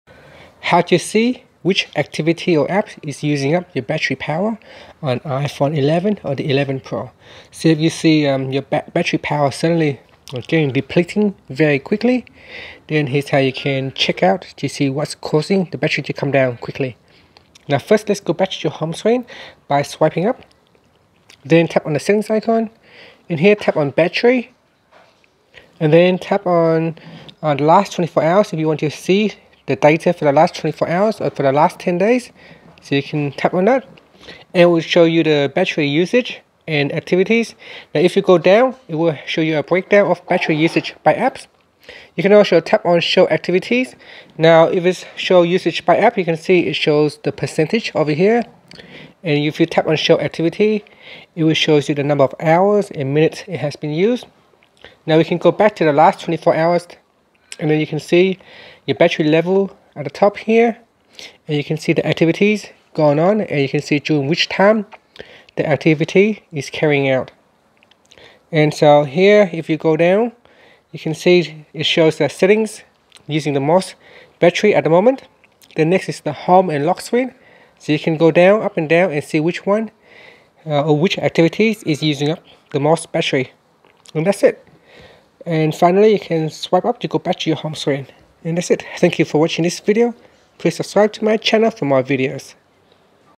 See Which Activities / Apps Using Up Battery Power on iPhone 11 Pro | iOS 13
See which activities or apps are using up the battery power on iPhone 11 Pro running iOS 13.

iOS 13.